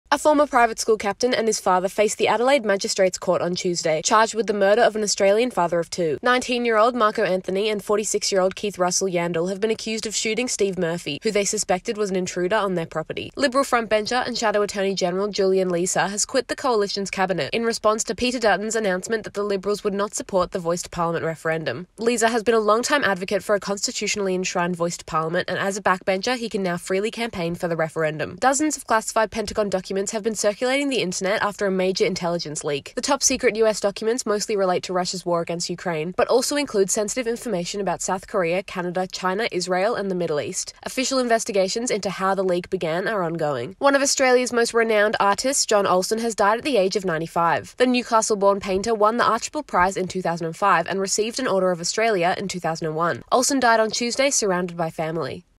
Shadow Attorney-General Julian Leeser quits Liberal frontbench | 12 April 2023 Yahoo News Breakdown
1. A former private school captain and his father faced the Adelaide Magistrates Court on Tuesday, charged with the murder of an Australian father-of-two. 19-year-old Marco Anthony and 46-year-old Keith Russell Yandle have been accused of shooting Steve Murphy, who they suspected was an intruder on their property.
2. Liberal frontbencher and shadow attorney-general Julian Leeser has quit the Coalition’s cabinet in response to Peter Dutton’s announcement that the Liberals would not support the Voice to Parliament referendum. Lesser has been a longtime advocate for a constitutionally enshrined Voice to Parliament and as a backbencher, he can now freely campaign for the referendum.
3. Dozens of classified Pentagon documents have been circulating the internet after a major intelligence leak. The top secret US documents mostly relate to Russia’s war against Ukraine, but also include sensitive information about South Korea, Canada, China, Israel, and the Middle East. Official investigations into how the leak began are ongoing.
4. One of Australia’s most renowned artists John Olsen has died at the age of 95. The Newcastle-born painter won the Archibald prize in 2005 and received an Order of Australia in 2001. Olsen died on Tuesday surrounded by family.